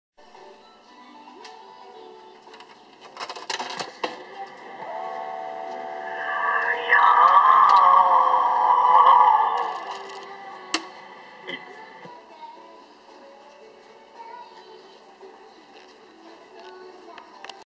ももねこ様のおみくじ
TVアニメ「たまゆら」の舞台となっている広島県竹原市、そこの駅前
商店街「あいふる316」に設置されている「ももねこ様おみくじ」という
のをやってみました。